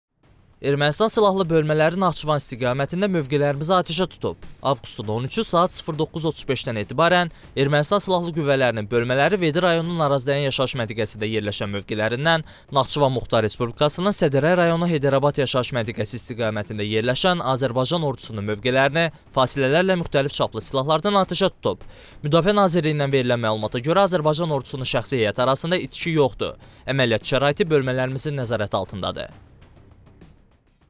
Naxçıvan atəşə tutuldu, Müdafiə nazirliyi açıqlama yaydı - Baku TV
Ermənistan silahlı bölmələri Naxçıvan istiqamətində mövqelərimizi atəşə tutub.

Avqustun 13-ü saat 09:35-dən etibarən Ermənistan silahlı qüvvələrinin bölmələri Vedi rayonunun Arazdəyən yaşayış məntəqəsində yerləşən mövqelərindən Naxçıvan Muxtar Respublikasının Sədərək rayonu Heydərabad yaşayış məntəqəsi istiqamətində yerləşən Azərbaycan Ordusunun mövqelərini fasilələrlə müxtəlif çaplı silahlardan atəşə tutub.

Müdafiə Nazirliyindən verilən məlumata görə, Azərbaycan Ordusunun şəxsi heyəti arasında itki yoxdur. Əməliyyat şəraiti bölmələrimizin nəzarəti altındadır.